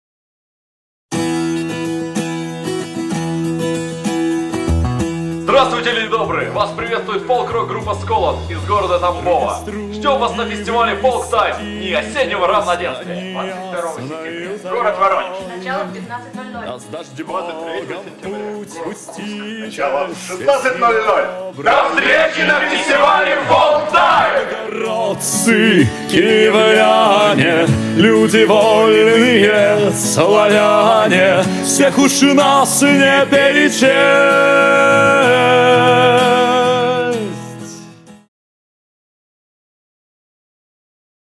FOLK TIME FESTIVAL 2012 - приглашение группы "СКОЛОТ" (Тамбов)
22 сентября - г.Воронеж (Паб "100 Ручьев"), начало - 15.00
23 сентября - г.Курск (ДК им.Железнодорожников), начало - 16.00